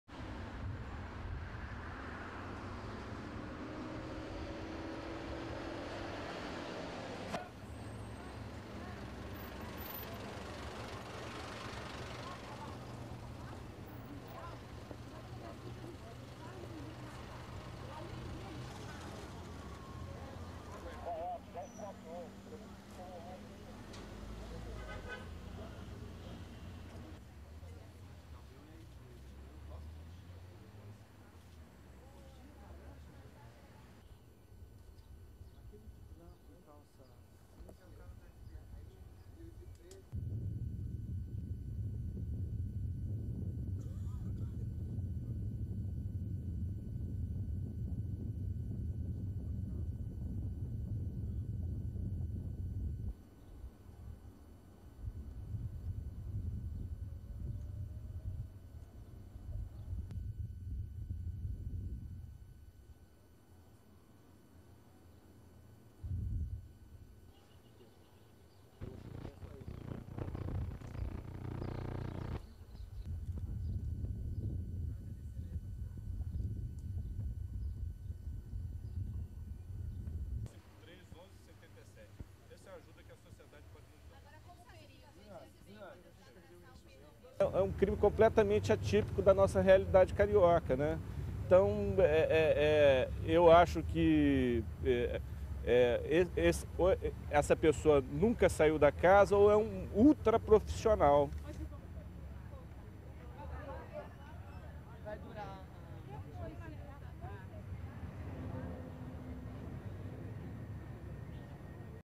Police reenact murder of Shell exec
1. Pan of avenue to entrance of condominium where press awaits
2. Police car, investigators enter condominium as camera follows police car
3. Car with military police officer inside entering condominium
4. Screen at condominium's security gate to zoom out of entrance
5. Various of investigators working behind mansion
6. Wide of mansion
7. More of investigators working behind mansion
8. Roger Ancellotti with reporters
9. SOUNDBITE: (Portuguese) Roger Ancellotti, Technical and Scientific Director, Rio de Janeiro State Civilian Police:
"This is an atypical crime by the standards of Rio de Janeiro...I think that...I think that this assassin never left the house or the crime was carried out by a super professional."
10. WS Roger Ancellotti enters condominium

STORYLINE:

Police re-enacting the brutal murder of a Shell oil executive and his wife said on Wednesday the killer was either highly professional or a member of the household.

American couple Todd and Michelle Staheli were found bludgeoned in their bed in a luxurious, high-security Rio de Janeiro condominium on Nov. 30 by their children.

Todd Staheli died on the scene and his wife died days later in the hospital.

The couple's four children and another American couple called in to help with the reenactment were not present, so police officers had to stand in for them.

A lawyer for the Staheli family said he does not want the children - aged between three and 13 - to return to the crime scene.

A maid and a driver worked in the house, but were not there on the night of the murder, police say.

Two FBI agents are in Rio to monitor the investigation.

The Staheli family had been in Rio de Janeiro for less than four months when the crime occurred.

Staheli, 39, vice president for joint ventures in the Southern Cone gas and power unit of Shell, was a native of Spanish Fork, Utah, and his wife was from Logan, Utah.

Although the homicide rate hovers around 50 per 100,000 residents in Rio, violence rarely spills into the city's high-security condominiums.